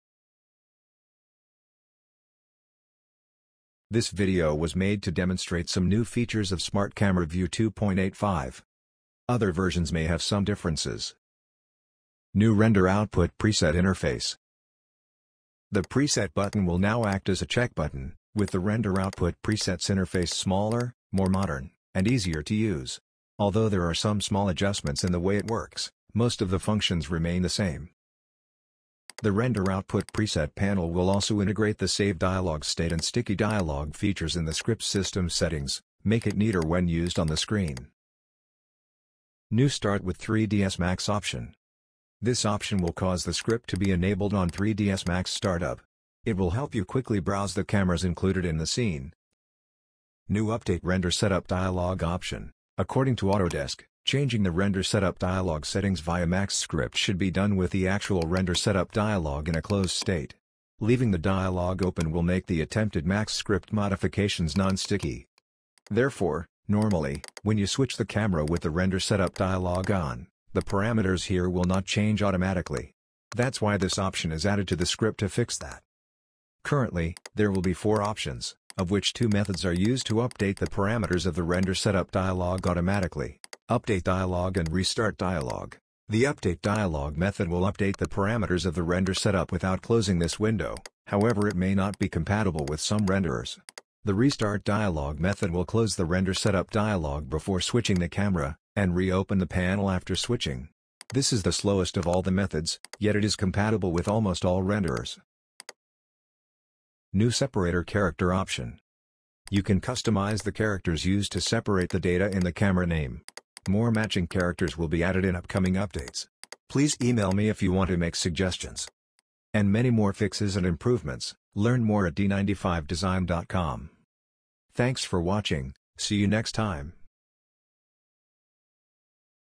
Smart Camera View 2.85 — Render Setup Update, New Preset Interface, Separator Character and more
SMART CAMERA VIEW 2.85 | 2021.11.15
Minor update to Smart Camera View 2, with some important additional features.

This script is intended to replace the default Camera View (C) in 3dsMax with some cool functions. It will save you a lot of time when setting camera, especially in scenes that has multiple cameras with different aspect ratios.